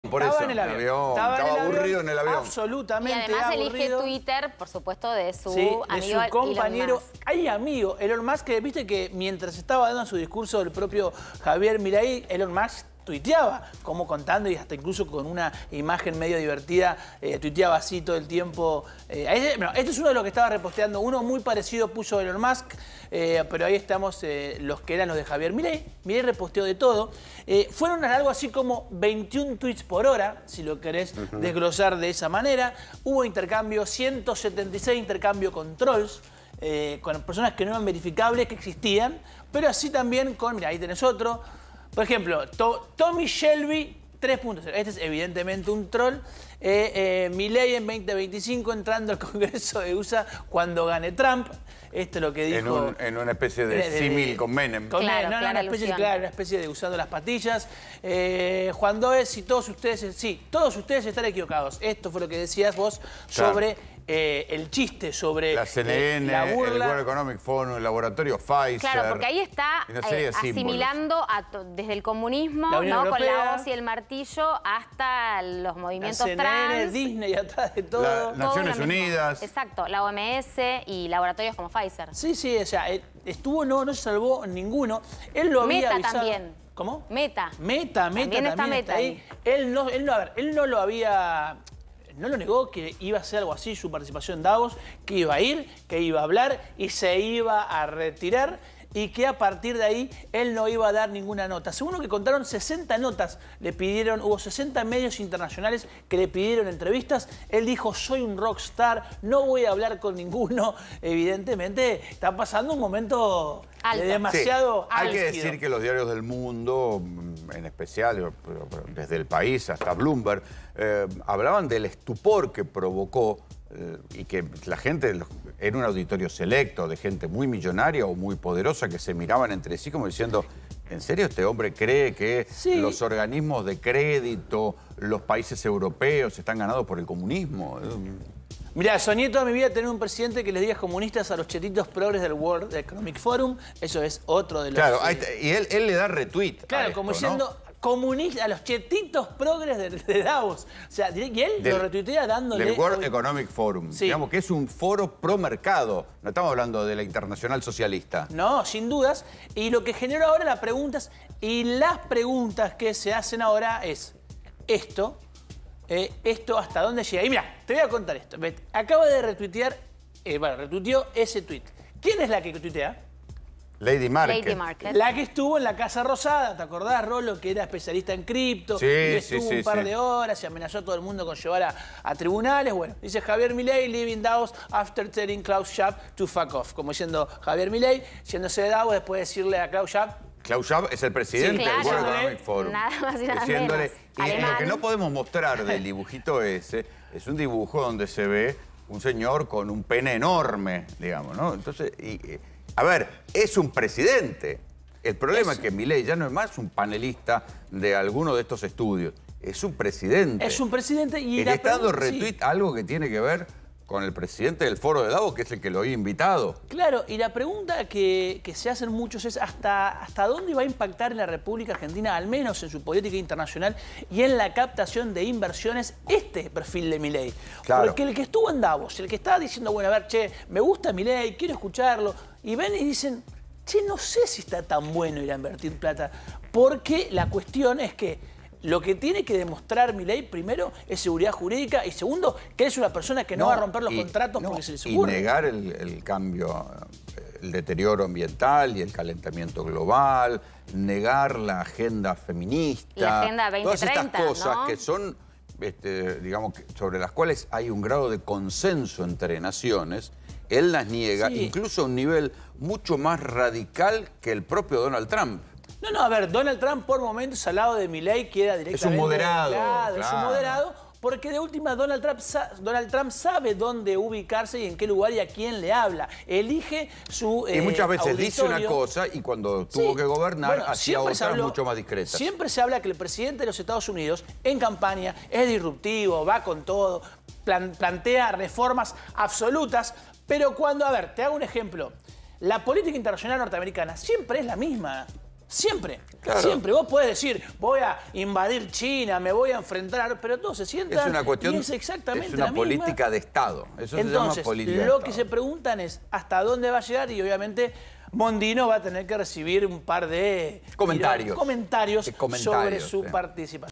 👀 LA NOCHE DE FURIA DE JAVIER MILEI: CATARSIS EN LAS REDES SOCIALES
📲 Milei tuiteó 517 veces en 24 horas.
✍ 60 medios le pidieron entrevistas.